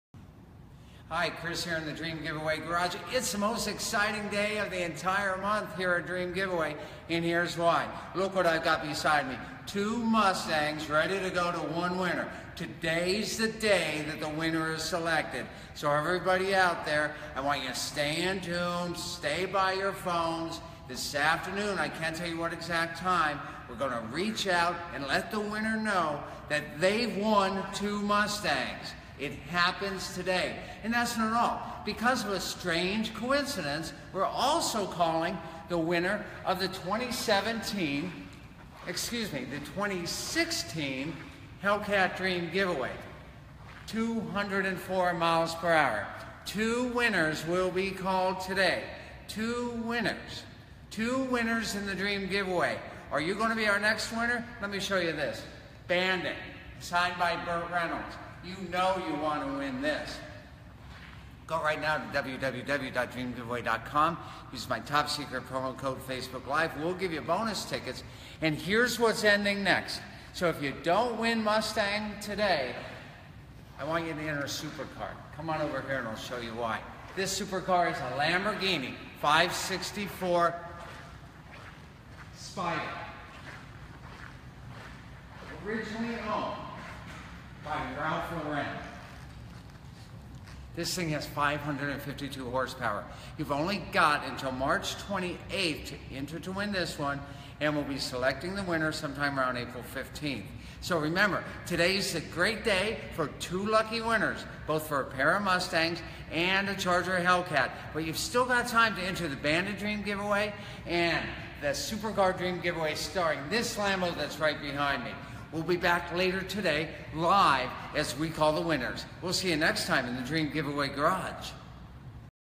Dream Giveaway - What's it Like Right Before We Say "You're the Winner?"
Have you ever waiting on phone for a national sweepstakes to call you and tell you that you're the winner. Here's what's going on in Dream Giveaway Garage right before the winners of 2 Shelbys and a Hellcat are notified. Originally broadcasted on Facebook live